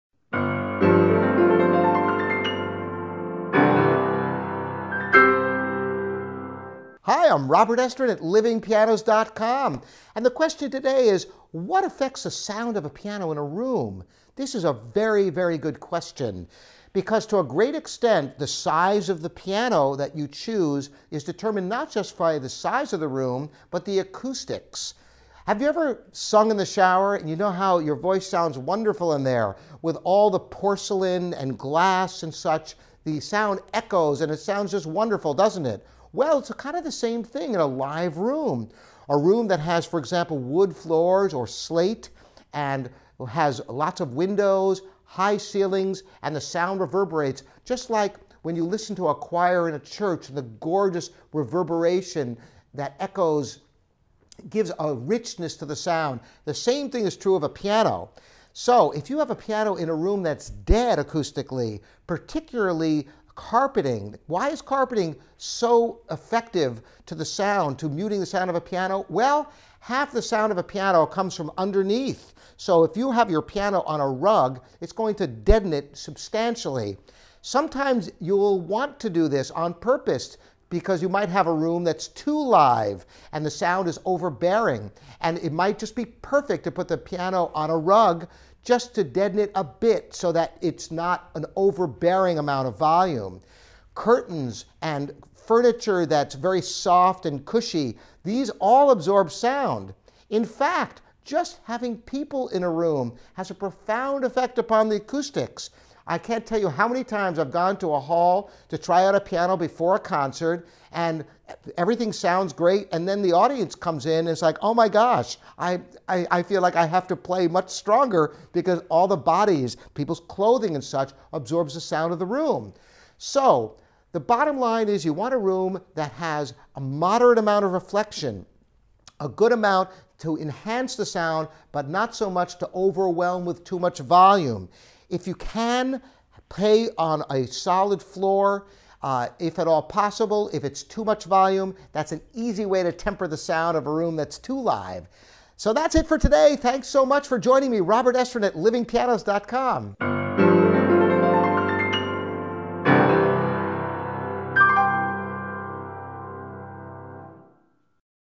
What Affects the Sound of a Piano in a Room? Acoustic Tips
This is a very good question. Many people might not realize that there are many different things that can affect the sound of a piano in a  room. Some good, some bad; but they are all things you should be aware of to determine the right size and placement of a piano in your home.

The size of the piano you choose for your home is very important. It is determined not only by the size of the room, but the acoustics. Have you ever sung in the shower and noticed how much better your voice sounds? Because of all the porcelain and glass, the acoustics allow your voice to be echoed off the walls and it has a pleasing effect. The same is true when it comes to any live room.

A room with wood floors or slate; lots of windows, and high ceilings may provide an ideal environment for a piano. The sound travels all around the room and reverberates very nicely. If you have a room that is dead acoustically, the piano won't sound nearly as pleasing. One of the biggest factors in a room is the floor; carpeting will dampen the sound of a piano. Why is this? Half of the sound of a piano comes from underneath the soundboard, so when the sound travels into the carpet or rug it will deaden the sound substantially.

Now there are occasions when you would actually want to deaden the sound of a piano. For example, if you have a room that is too live or has a bigger piano than it should, the sound of the piano can actually become overbearing. To solve this you can put a rug underneath the piano and it will dampen the sound a bit.

Other objects in a room that affect the sound of your piano are things like curtains and couches. Soft and cushy furniture will absorb sound and will actually affect the sound of your piano in the room. In fact, just having people in the room will affect the sound of a piano. When many people are in a room they will actually absorb some of the sound, making the piano just that much quieter. I can't tell you how many times I've gone to a hall before a concert and tested out the piano only to find it sounded completely different once the seats were filled for the actual performance.

The bottom line is that you want a room that will enhance the sound of the piano. You don't want the sound to be too loud or too quiet. Obviously this can be a challenge if you already have a piano in a certain room and it will be difficult to change anything but little modifications can help to improve the sound of your room. Some people will even put clear plexiglass under the piano if it is sitting on carpeting to help reflect the sound projecting under the piano!